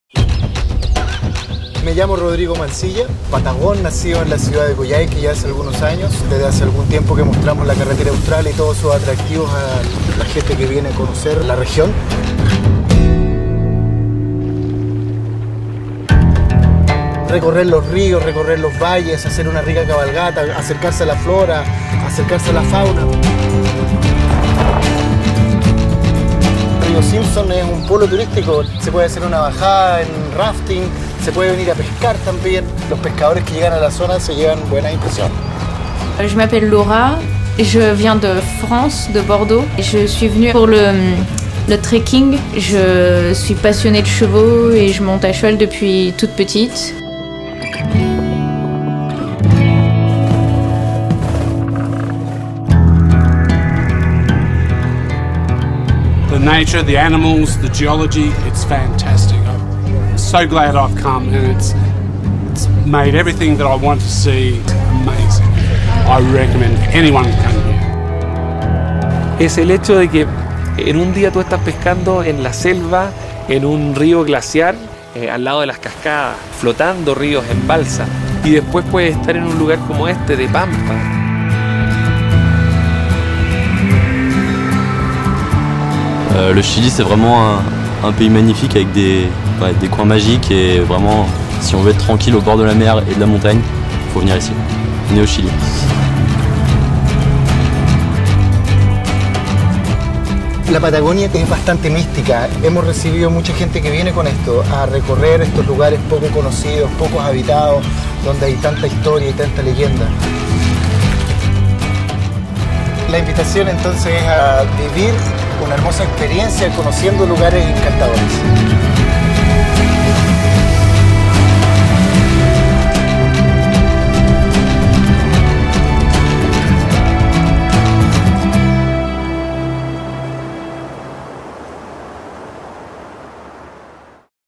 Qué hacer en Chile: Soft Adventure Patagonia - Naturaleza Abierta
Patagonia es una tierra única, donde cada paisaje invita a conectarse profundamente con esa naturaleza indómita y conmovedora. Bosques milenarios, poderosos ríos y glaciares monumentales que debes explorar y sentir. La Patagonia va contigo siempre, una vez que la visitas.